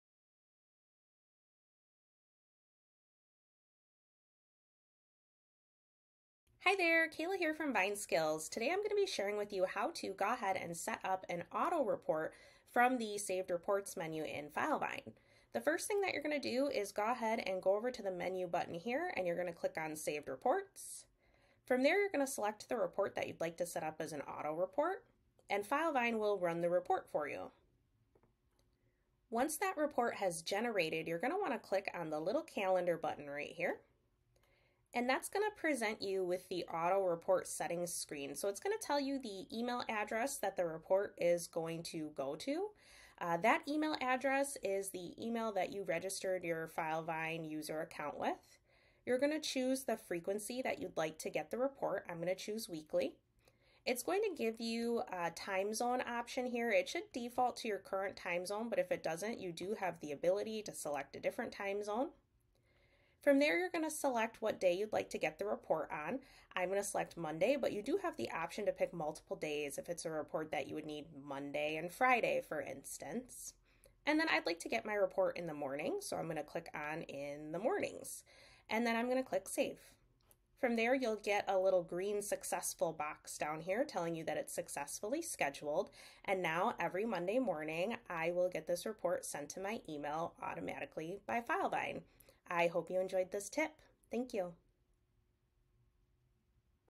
How to Set Up Filevine Auto Reports from Saved Reports
This video shares with the user how to set up auto-reports from the saved reports screen in Filevine.